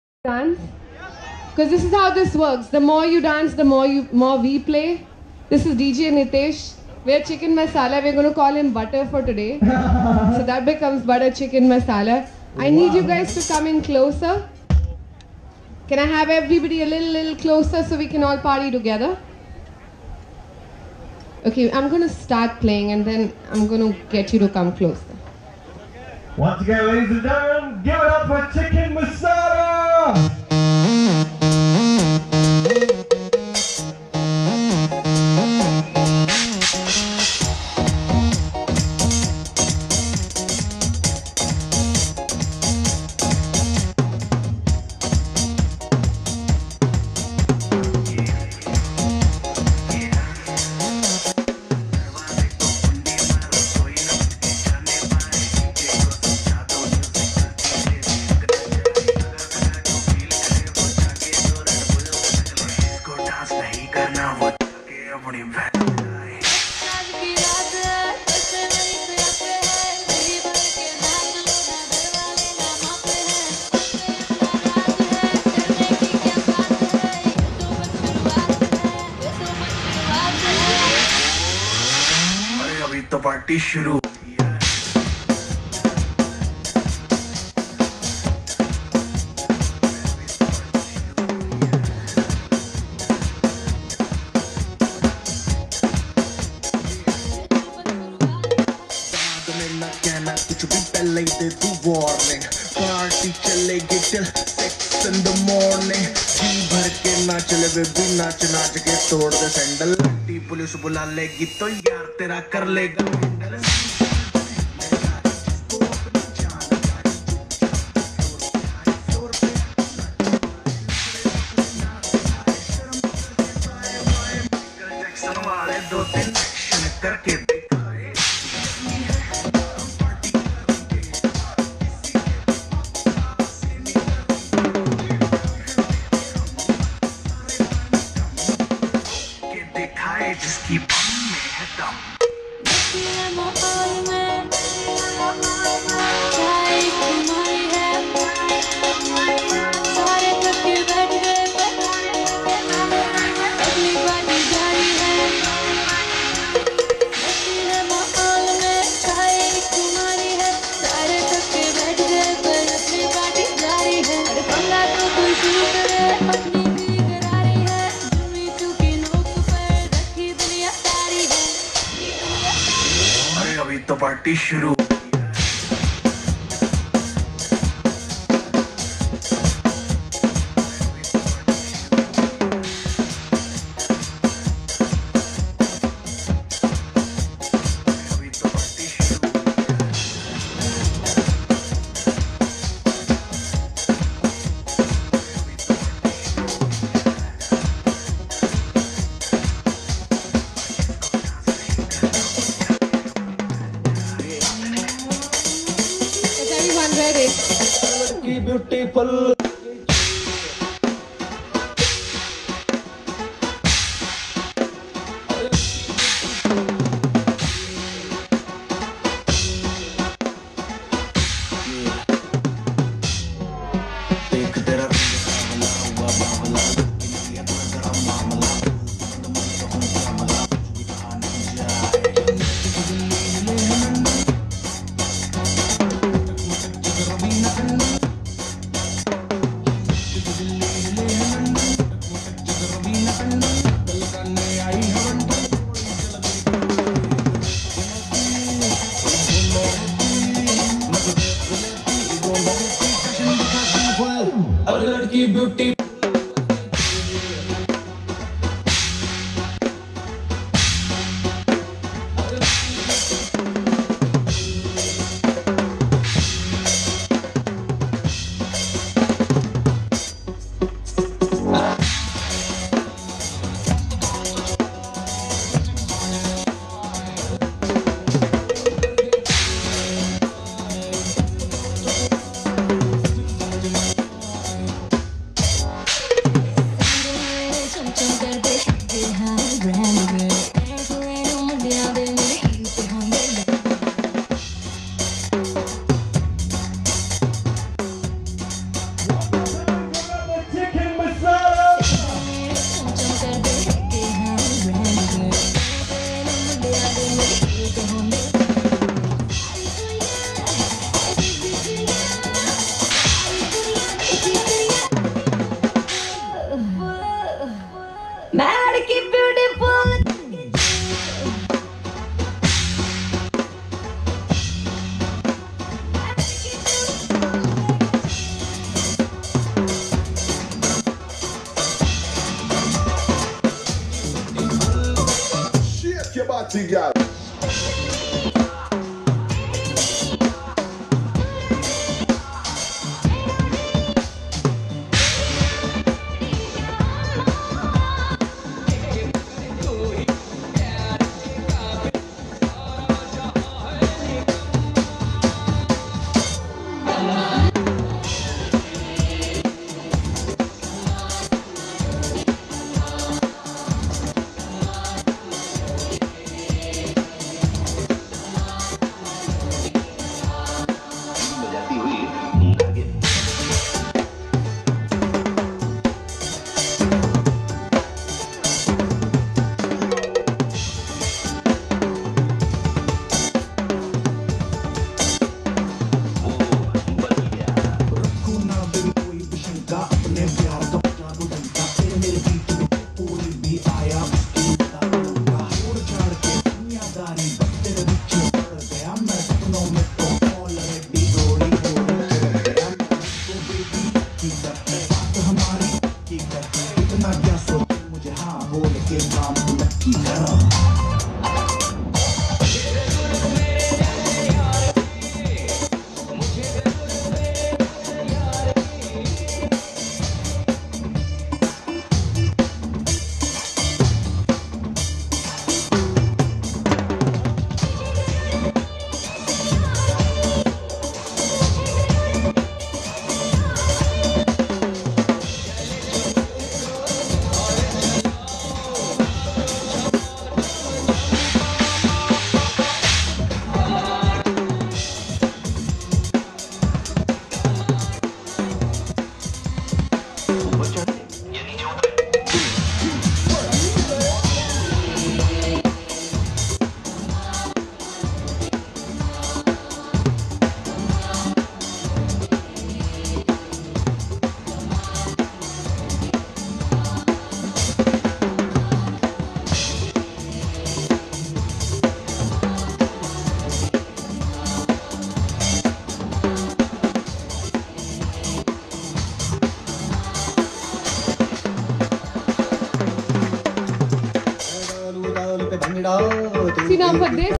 Bollywood Jam / Chickeen Masala live in Dubai
The start of the set on the beach at the Fairmont Ajman hotel UAE